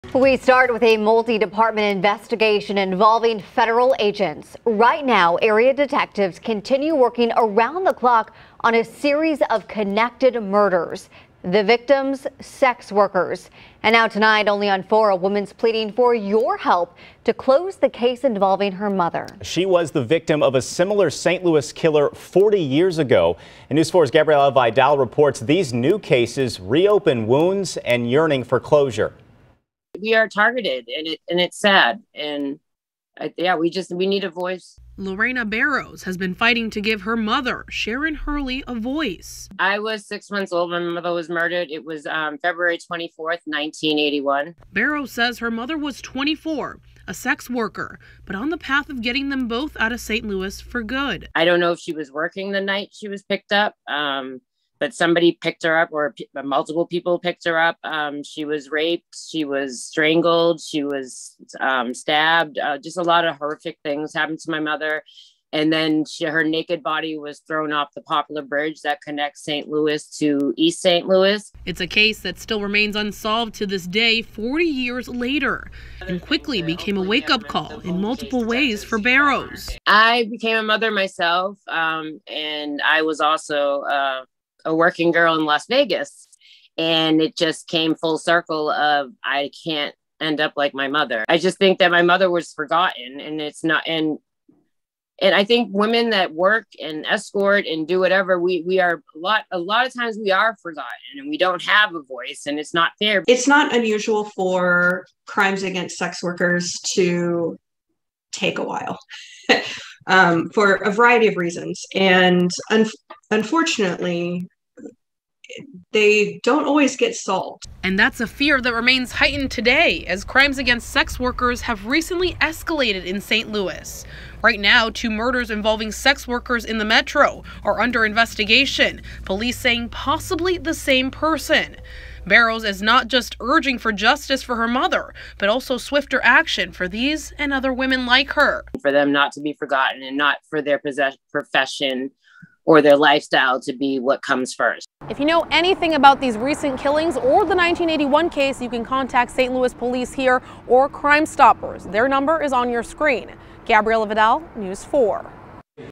Former St. Louis resident looking for justice in mother's cold case, other sex worker crimes
"I don't know if she was working the night she was picked up, but somebody picked her up … or multiple people picked her up,” Barrows said. “She was raped, she was strangled, she was stabbed. Just a lot of horrific things happened to my mother. And then her naked body was thrown off the Poplar Bridge that connects St. Louis to East St. Louis."